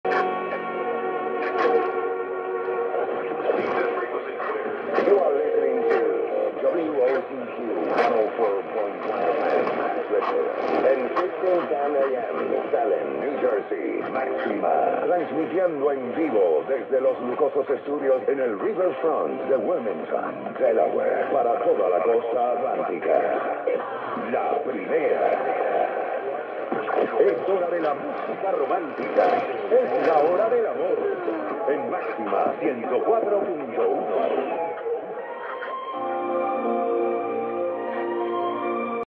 WOCQ 1510 AM Salem, NJ (05.12.2024)
Call Letters & Frequency: WOCQ 1510 KHz
Branding: Máxima 104.1
Transmitter distance: 114 miles (183 km)
Daytime power: 2,500 watts
Nighttime power: None
Location received: Clifton, New Jersey, USA
Date & Time (EDT): Sunday, May 12, 2024 at 11:50 PM
Date & Time (UTC): Monday, May 13, 2024 at 03:50
Content: Legal ID, Station Sweeper

WOCQ was broadcasting even though it was supposed to sign off at sunset.  It also operates an FM translator (104.1 W281CM Millville, New Jersey).